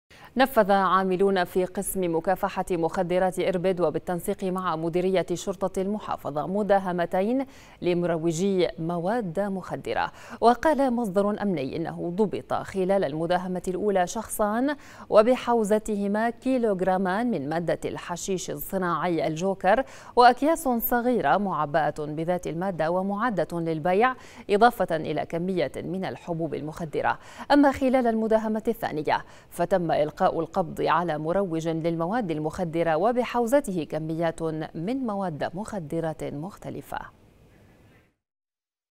مداهمة مجموعة من مروجي المواد المخدرة بحوزتهم كليو غرامان من الحشيش والجوكر - (22-5-2018)
نفَّذَ عاملون في قسمِ مكافحةِ مخدراتِ إربد و بالتنسيقِ مع مديريةِ ‏شرطةِ المحافظة مداهمتيْن لمروجي موادَّ مخدِرة.
و قالَ مصدرٌ أمنيٌ إنَّه ضُبطَ خلالَ المداهمةِ الأولى شخصان و بحوزتهما كيلو غرامانِ من مادةِ الحشيشِ الصناعي الجوكر و أكياسٌ صغيرةٌ معبأةٌ بذاتِ ‏المادة و معدةٌ للبيع، إضافةً إلى كميةٍ من الحبوبِ المخدرة، أمّا خلالَ ‏المداهمةِ الثانية فتمَّ إلقاءُ القبضِ على مروجٍ للموادِ المخدرة و بحوزته ‏كمياتٌ من موادَّ مخدرةٍ مختلفة.‏